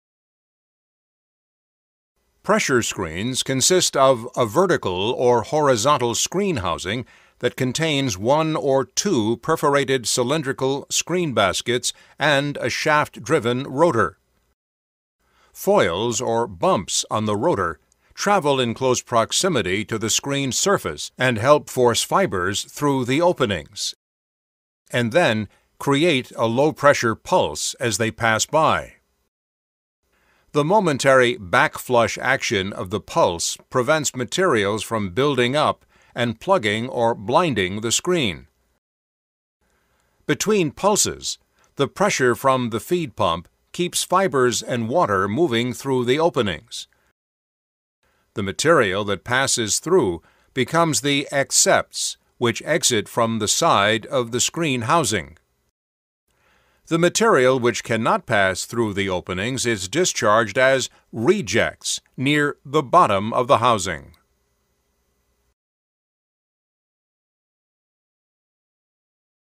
Thin Stock Screening
to view the full video and purchase access to our other Paper courses.

The purpose of thin stock screening is to remove debris from the dilute stock going to the headbox. Screening also deflocculates the stock and protects downstream equipment. Screening systems often use two or three "cascaded" stages in order to reduce fiber losses and improve overall debris removal efficiency. The design and operation of a screening system must take into account the requirements of the paper or board machine, desired sheet quality and properties, and characteristics of the particles to be removed.